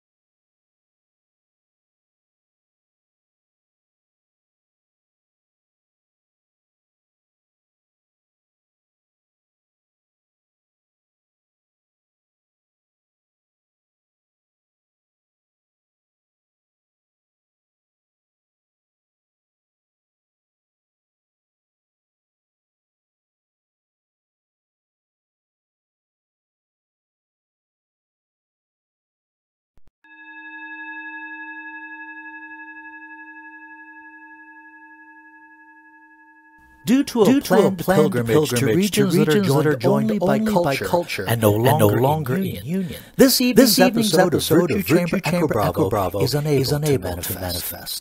Anthony Law & Philip Nichol Talking Live With Moving Pictures Behind Their Heads
Join Anthony Law & Philip Nichol for chat, discussion, deliberation, musings, chinwags and  discourse.  This is Virtue Chamber Echo Bravo.  It is time.